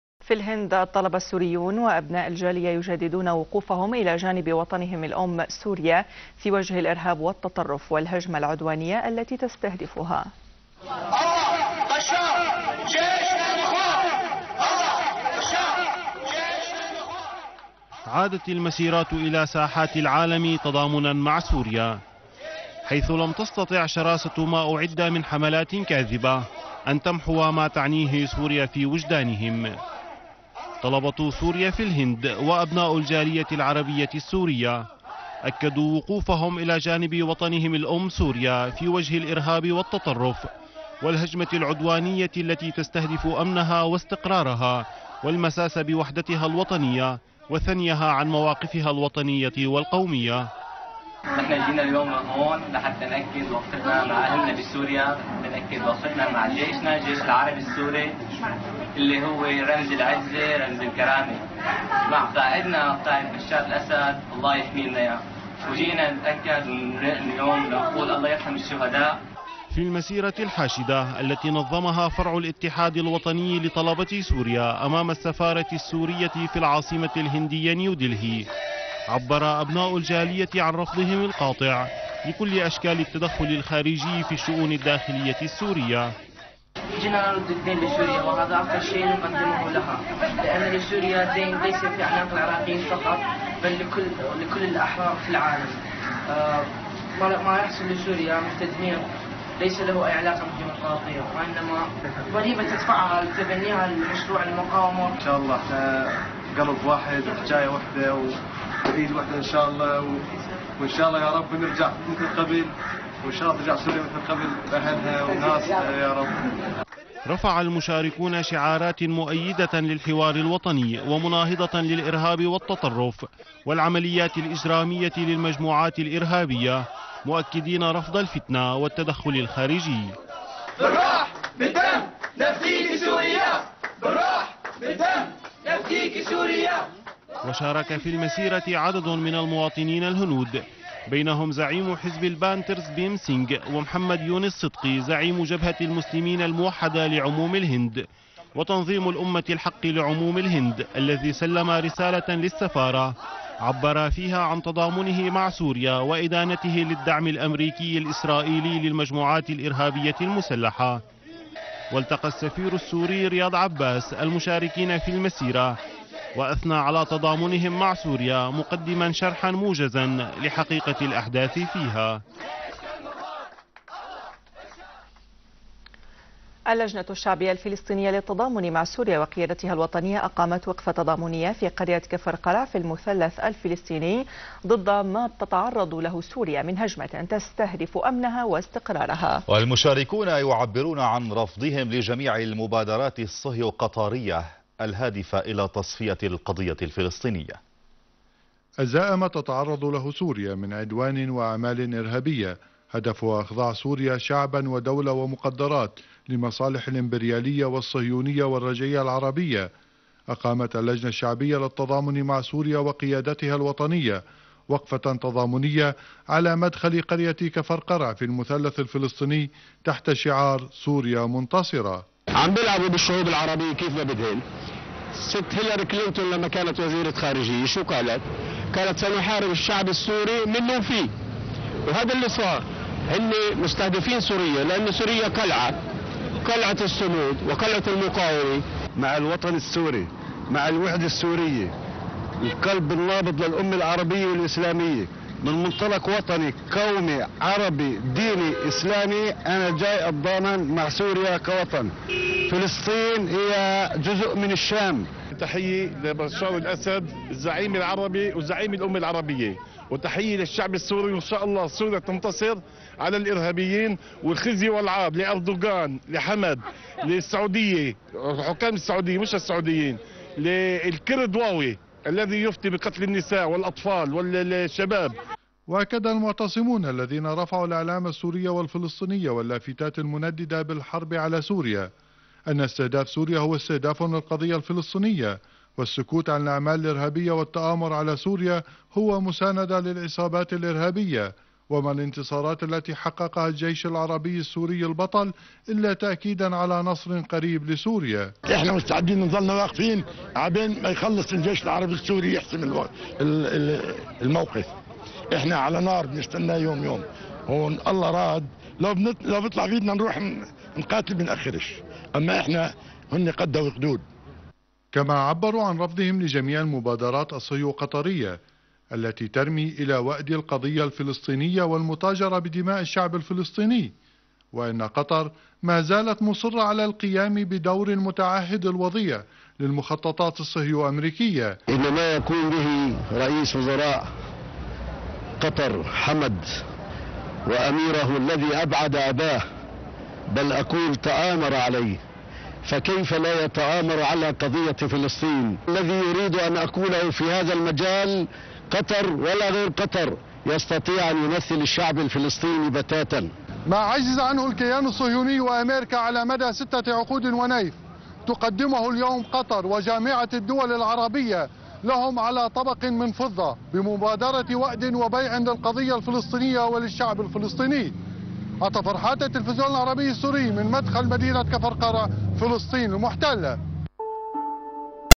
SYRI TV | وقفة تضامنية للطلبة في الهند وفلسطين ضد ما تتعرض له سوريا 03 - 05 - 2013
شبكة أخبار قمحانة , Syrian Truth | الحقيقة السورية